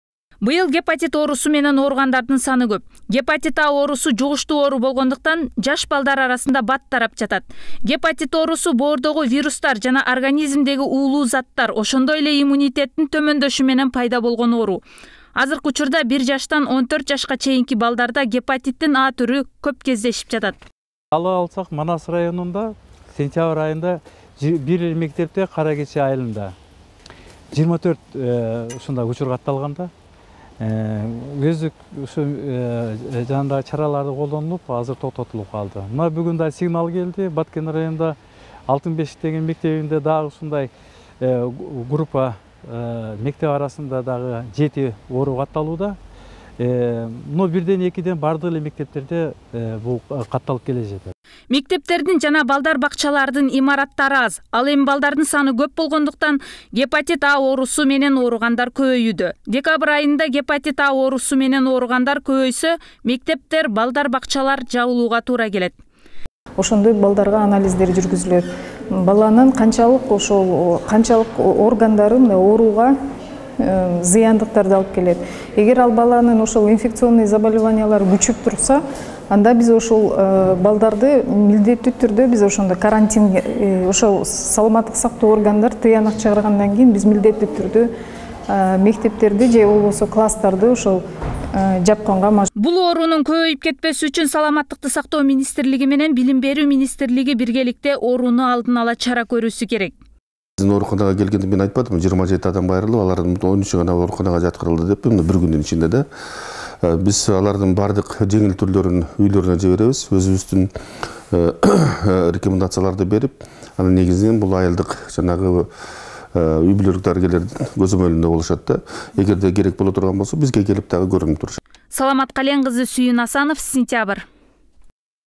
"Гепатит А" оорусу менен ооруган жаш балдардын саны күн санап өсүүдө
ТРКС С-2 СЕНТЯБРЬ ТВ